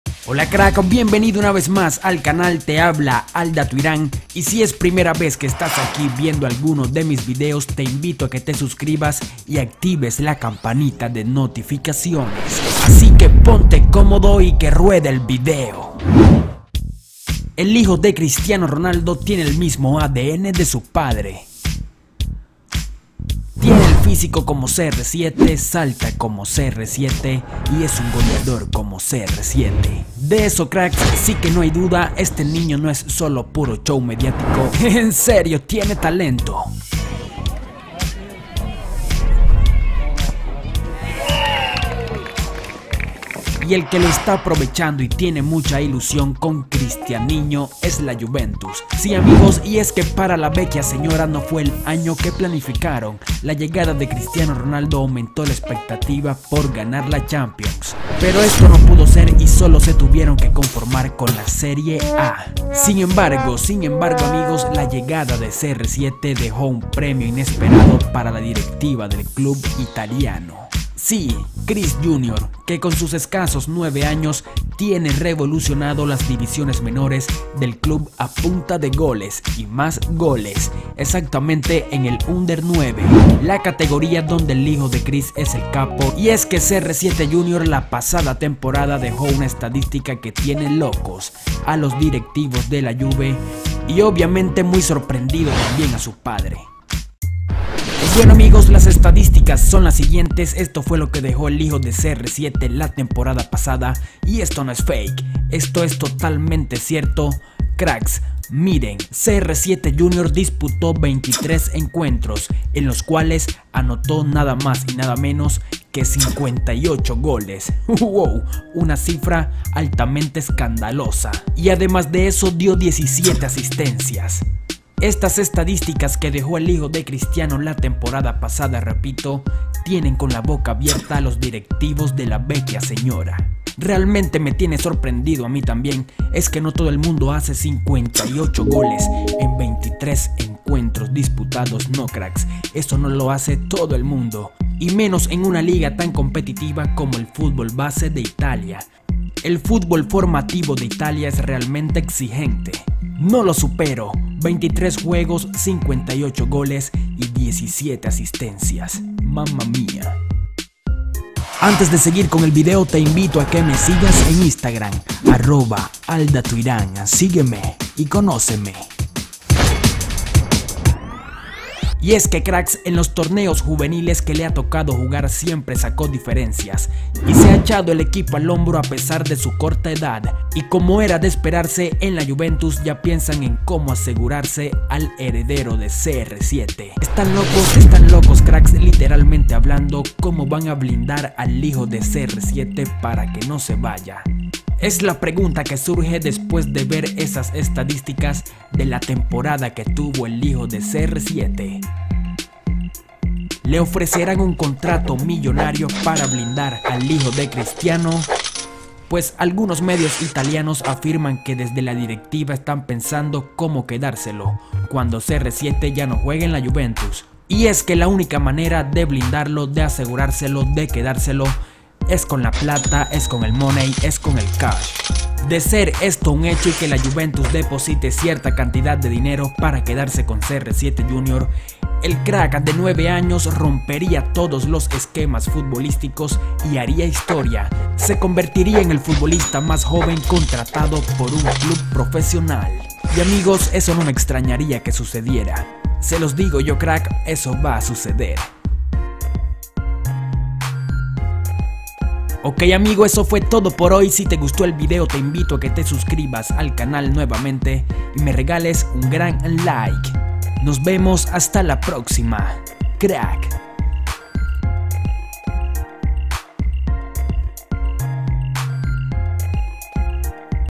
HIJO DE CR7 MARCÓ 58 GOLES EN LA TEMPORADA Y LA JUVE QUIERE COMPRARLO PARA EL FUTURO 😱
El hijo de Cristiano Ronaldo, tiene le mismo ADN  de su padre, tienen el físico como CR7, salta como CR7, y es un goleador como CR7, la llegada de CR7 dejó un premio inesperado para la directiva del club italiano. Si, cris junior, que con sus escasos 9 años tiene revolucionado las divisiones menores del club a punta de goles, LA TEMPORADA ANTERIOR ANOTÓ 58 GOLES Y TIENE PENSANDO  A LOS DIRECTIVOS DEL CLUB. ¿LO COMPRARÁN?

🔔RECIBE NOTIFICACIONES CADA VEZ QUE PUBLICO UN NUEVO VÍDEO:
2. Dale Check a: "Recibir todas las notificaciones de este canal"
3. Dale a guardar